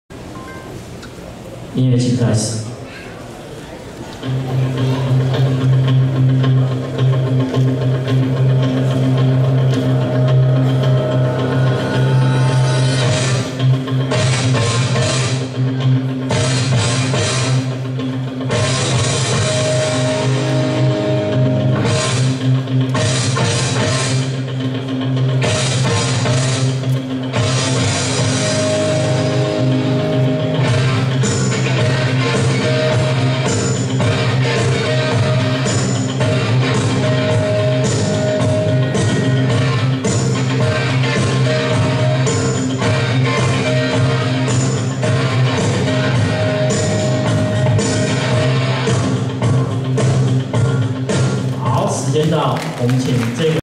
2014 全國總統盃 社會組 -85KG 第三名 114王遷聖
2014 全國總統盃 社會組 -85KG 第三名 114王遷聖
冠軍 111吳金龍
第二名 109劉冠忠
第三名 114王遷聖
第四名 107陳弘彬
第五名 110黃正賢
第六名 113鐘承臻